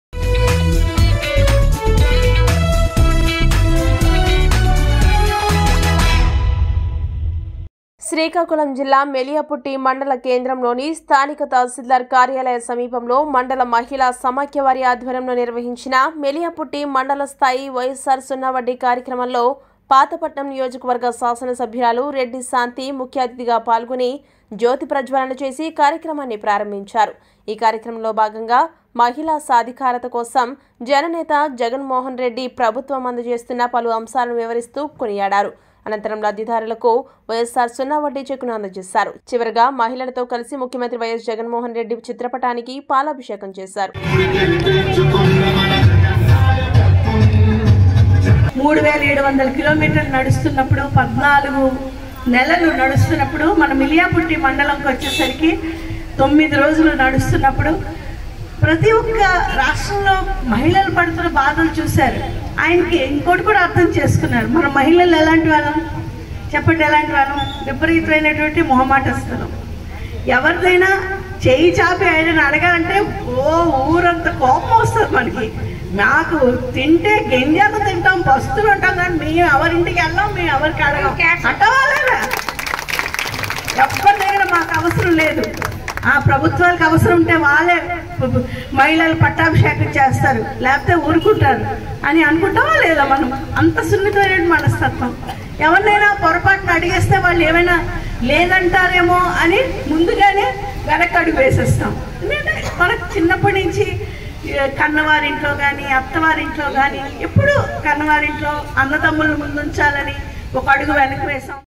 శ్రీకాకుళం|| YSR సున్నా వడ్డీ కార్యక్రమంలో, పాతపట్నం MLA రెడ్డి శాంతి పాల్గొన్నారు..
@truthnewstelugu శ్రీకాకుళం|| YSR సున్నా వడ్డీ కార్యక్రమంలో, పాతపట్నం MLA రెడ్డి శాంతి పాల్గొన్నారు..